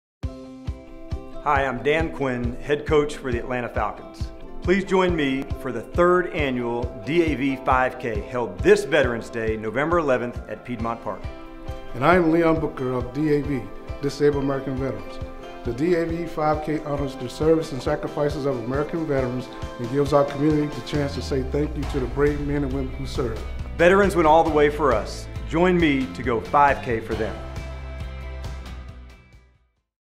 Falcons Head Coach Dan Quinn - DAV5K
Atlanta Falcons Head Coach Dan Quinn  and Disabled American Veteran Leon Booker Invite you to run, walk, roll, or ride a motorcycle for the Atlanta DAV5K.